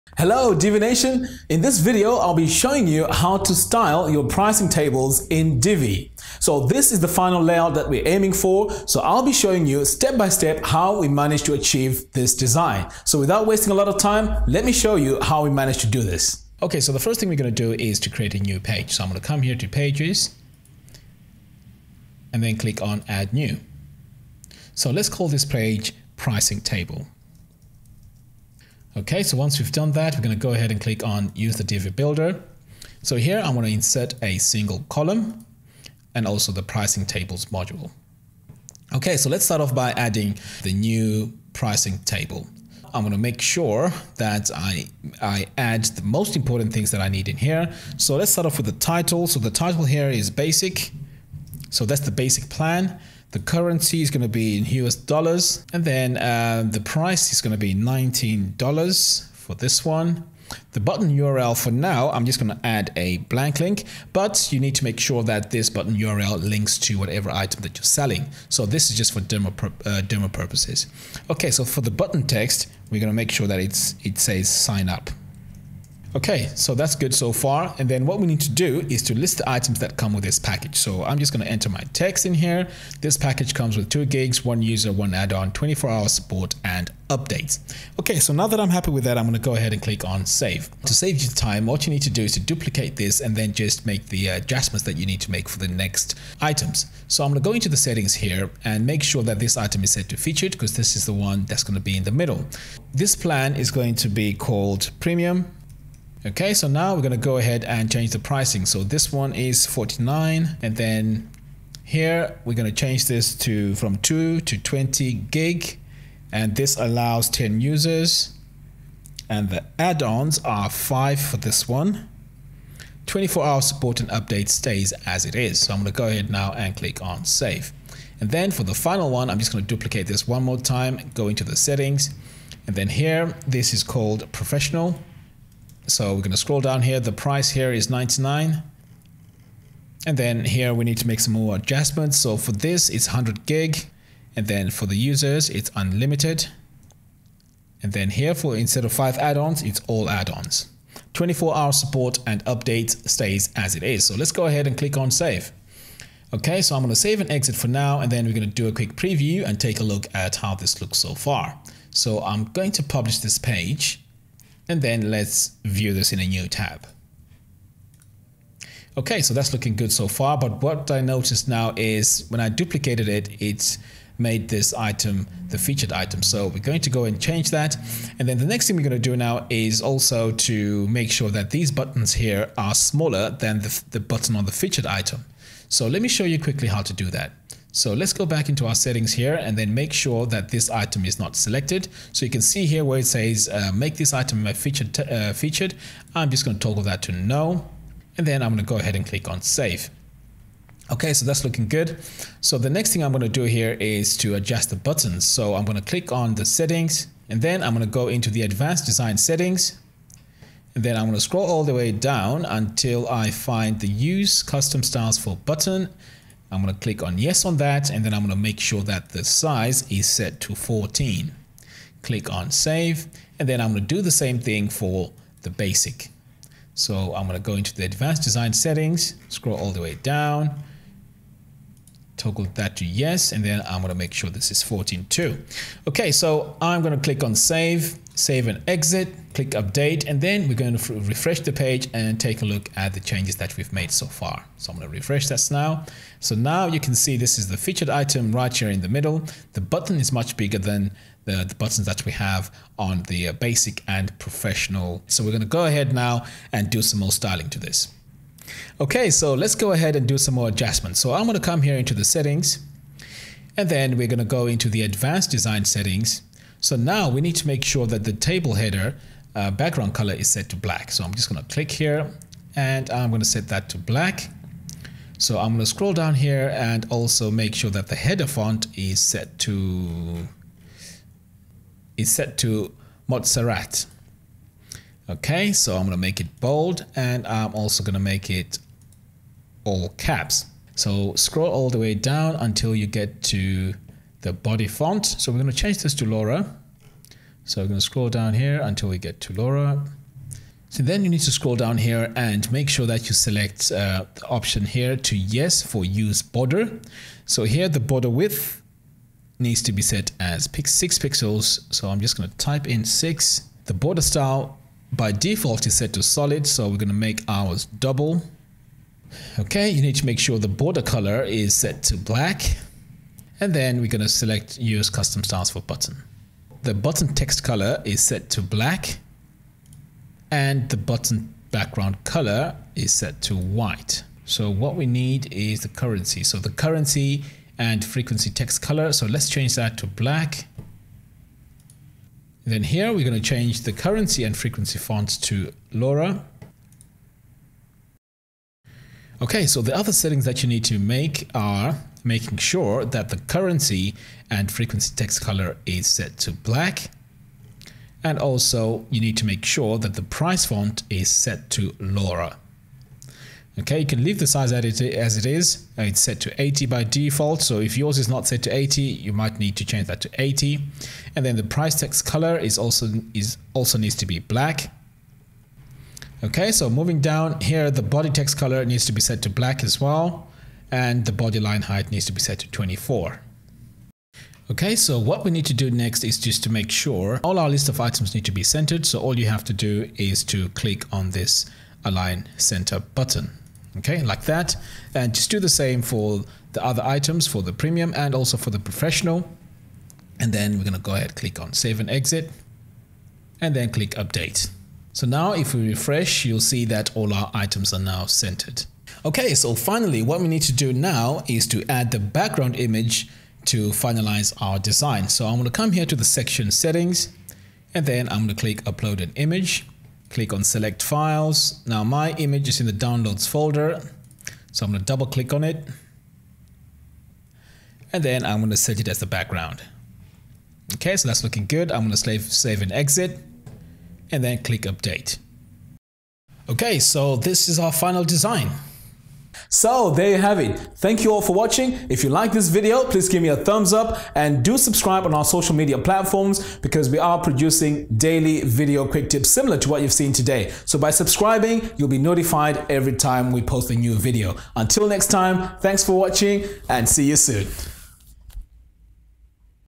How to Style a Beautiful Pricing Table in Divi
Pricing tables can be a hugely important element for converting a potential customer into a full-fledged buyer. While your pricing table has a very specific role within your site (that of providing information), it also offers the opportunity to catch the eye with its design. Simply put, an attractive pricing table can help to captivate a buyer, and even persuade them to make a purchase.